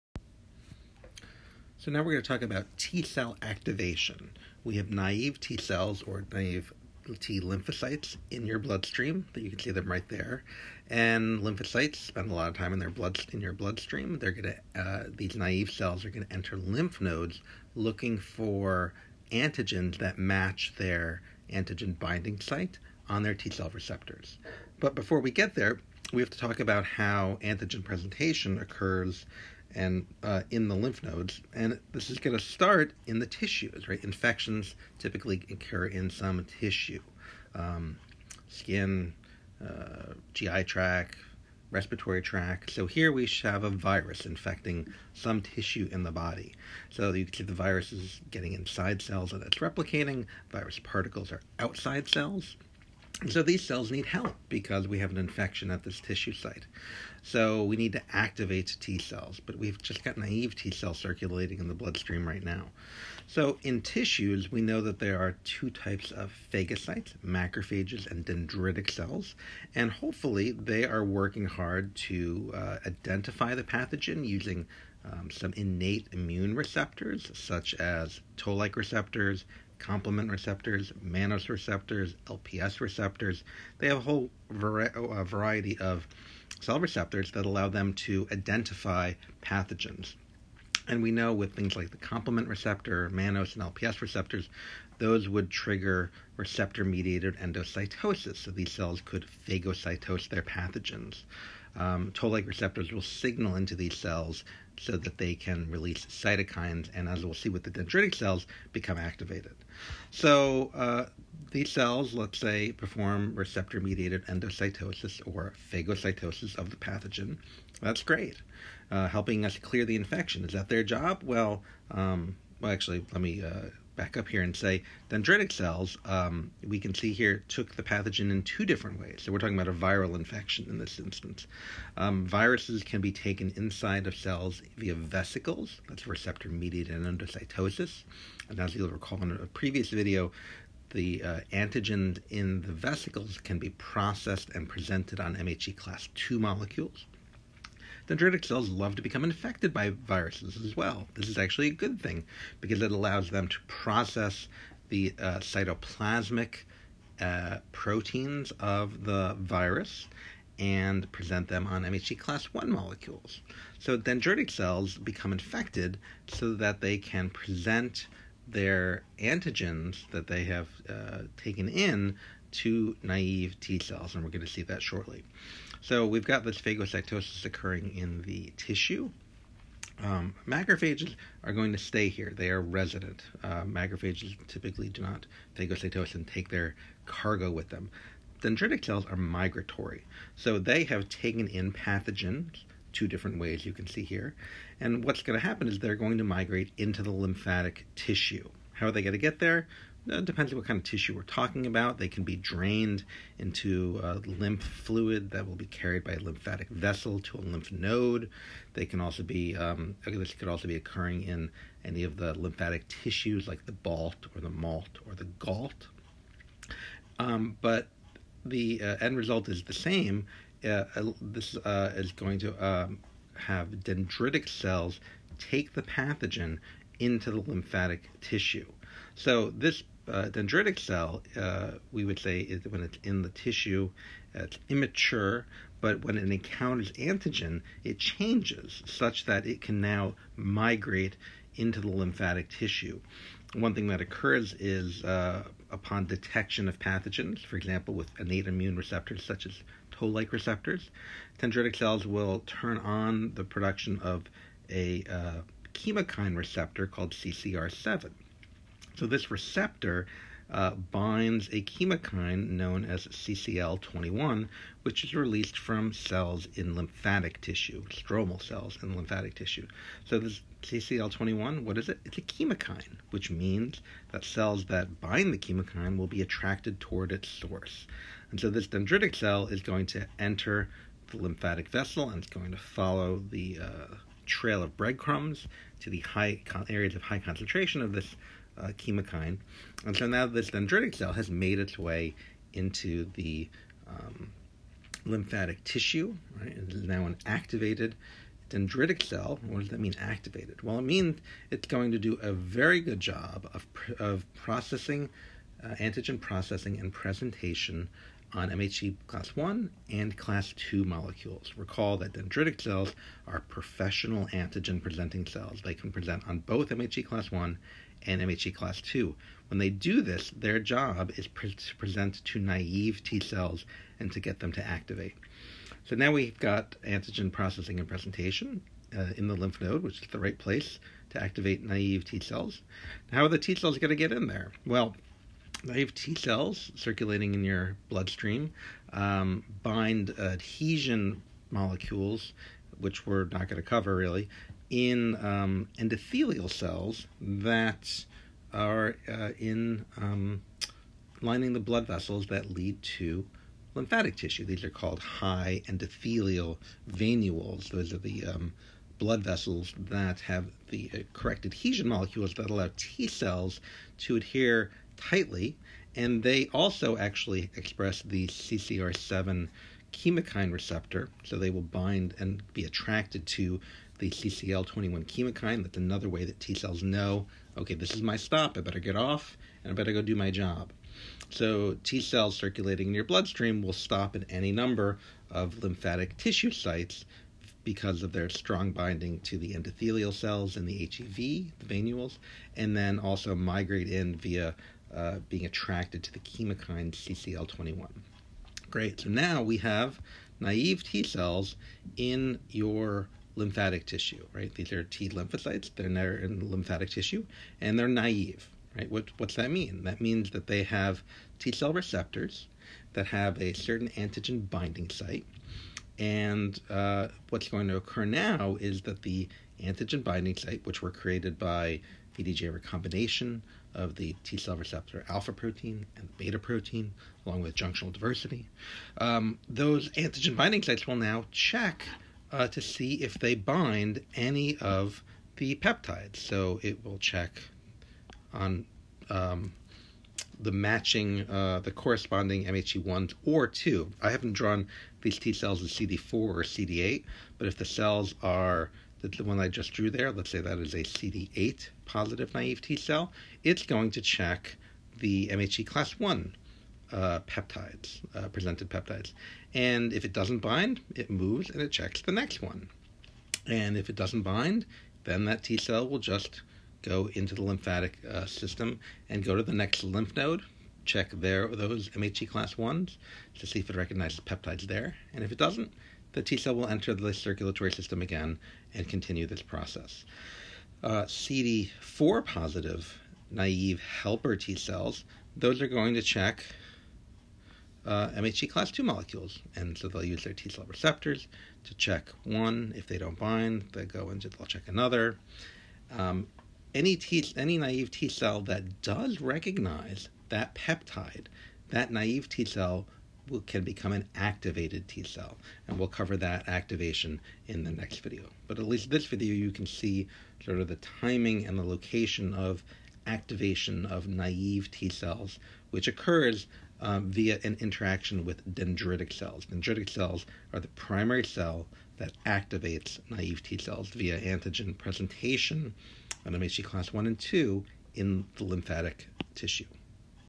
Activation of Naive T cells - from tissue to lymph node
In this video, I present how naive T cells become activated T cells in the lymph nodes.